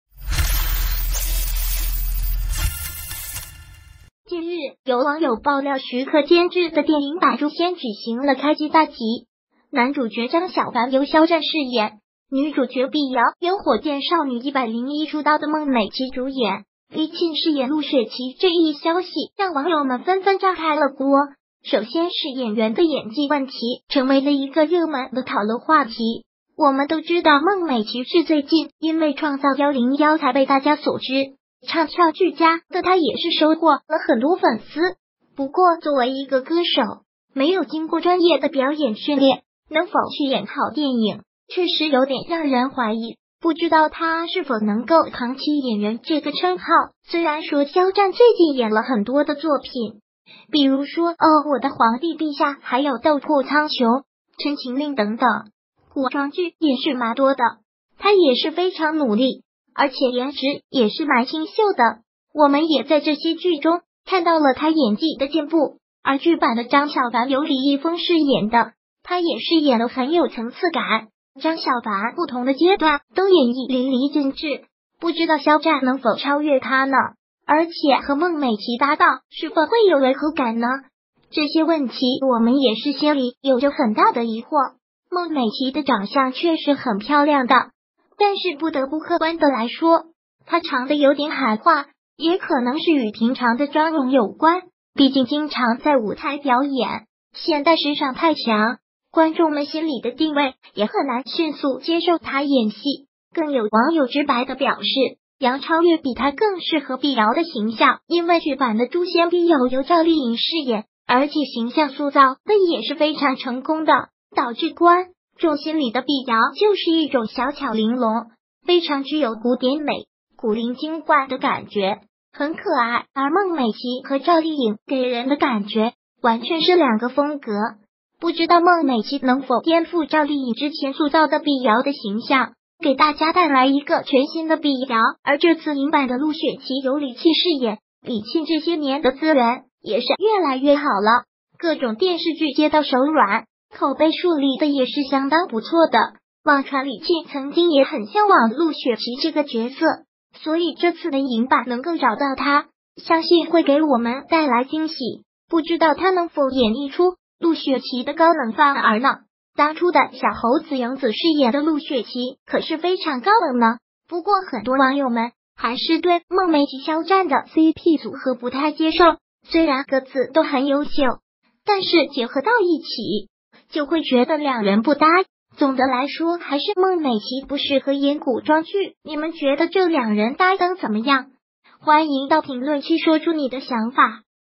诛仙电影剧照曝光，孟美岐演碧瑶，肖战演张小凡；这搭档你期待吗
近日有网友爆料徐克监制的电影版诛仙举行了开机大吉，男主角张小凡由肖战饰演，女主角碧瑶由火箭少女101出道的孟美岐主演，李沁饰演陆雪琪这一消息让网友们纷纷炸开了锅，首先是演员的演技问题成为了一个热门的讨论话题，我们都知道孟美岐是最近因为创造101才被大家所知，唱跳俱佳的她也是收获了很多粉丝，不过作为一个歌手，没有经过专业的表演训练，能否去演好电影，确实有点让人怀疑。不知道她是否能够扛起演员这个称号虽然说肖战最近演了很多的作品，比如说哦我的皇帝陛下还有斗破苍穹，陈情令等等，古装剧也是蛮多的，他也是非常努力，而且颜值也是蛮清秀的，我们也在这些剧中看到了他演技的进步，而剧版的张小凡由李易峰饰演的，他也是演的很有层次感，张小凡不同的阶段都演绎淋漓尽致，不知道肖战能否超越他呢？而且和孟美岐搭档是否会有违和感呢，这些问题我们也是心里有着很大的疑惑。孟美岐的长相确实很漂亮的，但是不得不客观的来说，她长得有点“韩化”，也可能是与平常的妆容有关，毕竟经常在舞台表演，现代时尚太强，观众们心里的定位也很难迅速接受她演戏，更有网友直白的表示，杨超越比她更适合碧瑶的形象因为剧版的诛仙碧瑶由赵丽颖饰演，而且形象塑造